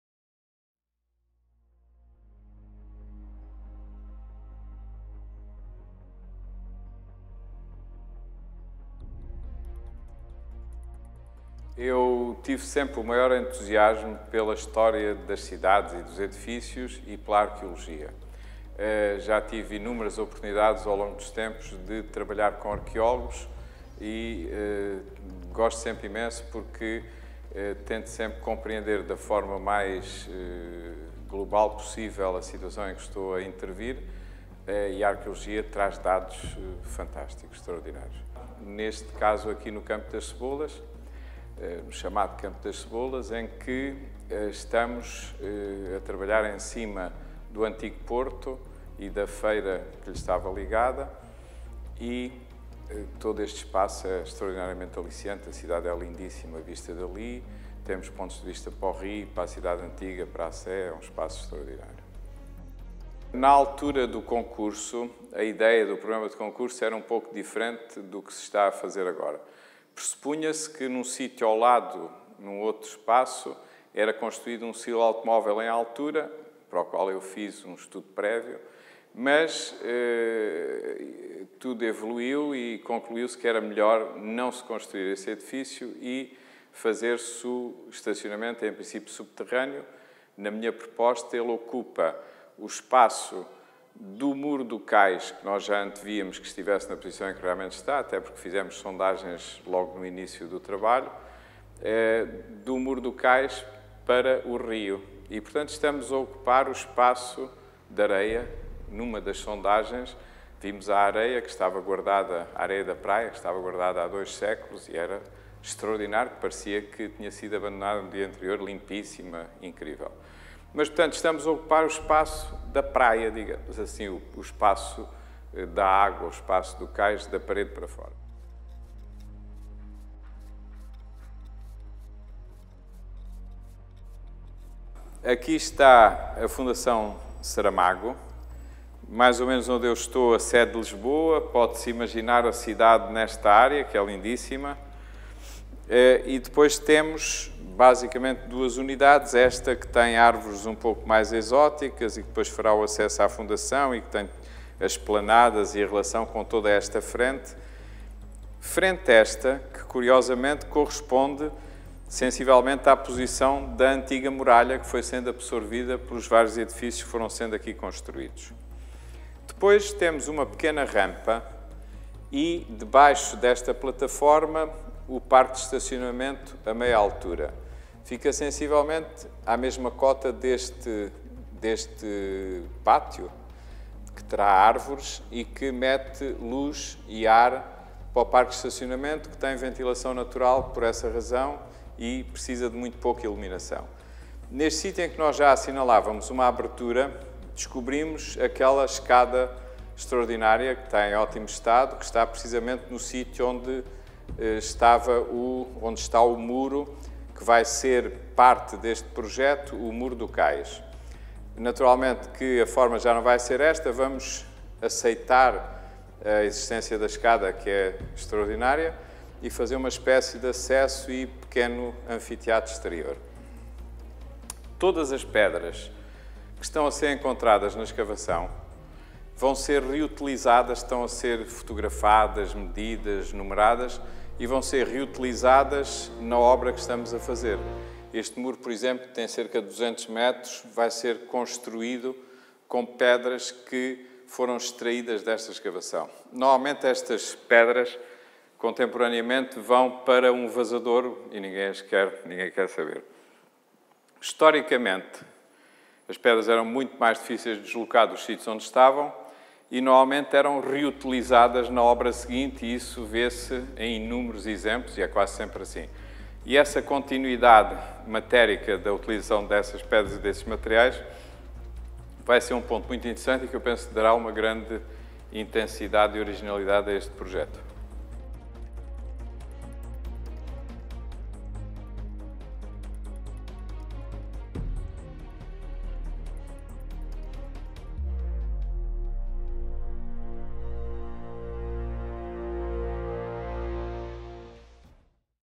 O novo Campo das Cebolas - O projecto de arquitectura | The new Campo das Cebolas square (3)
Sobre a saudável relação entre arqueologia e arquitectura, nas palavras do arquitecto Carrilho da Graça a propósito do seu projecto para o novo Campo das Cebolas, em Lisboa.
Mais uma grande escavação arqueológica da ERA!

On the healthy relationship between archaeology and architecture, in the words of architect Carrilho da Graça on his project for the new Campo das Cebolas in Lisbon.
Another great archaeological excavation by ERA!